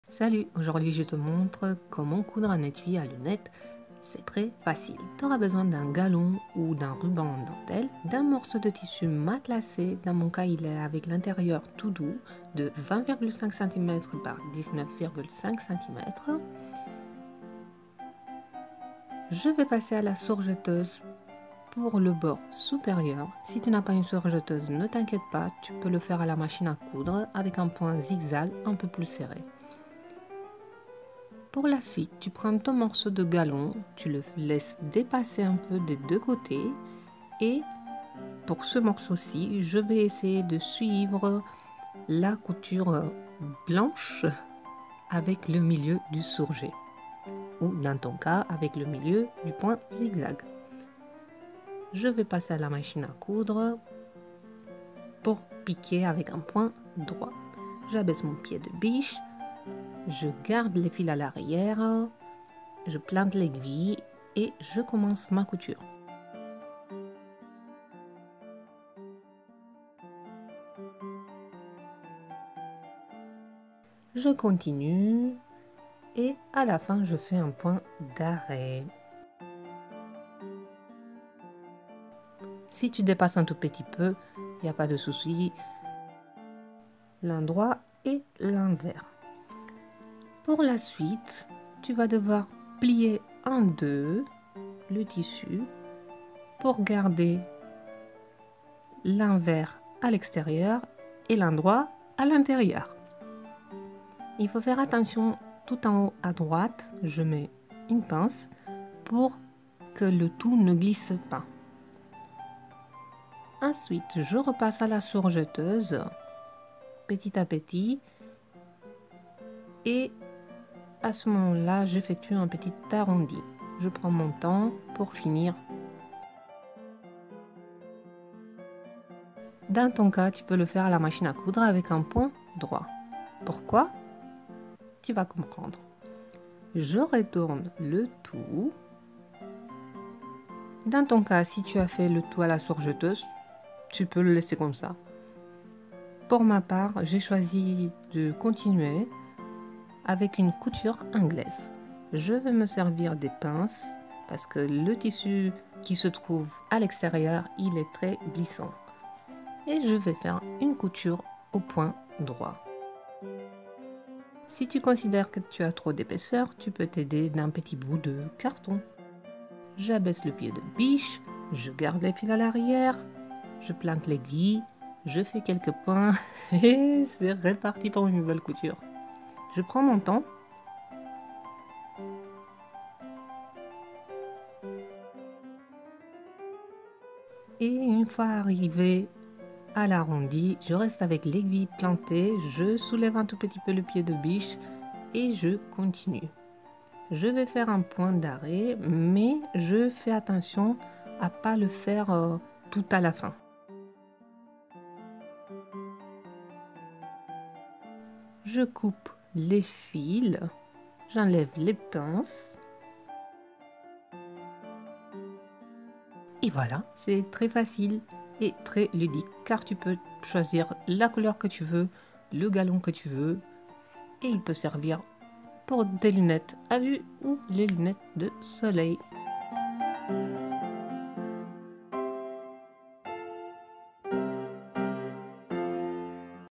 Tuto couture facile - étui à lunettes - DIY débutant
Comment coudre un étui à lunettes avec un seul morceau de tissu matelassé double face ?
Un projet couture si facile et utile que tu voudras essayer. ;)

Bisous :*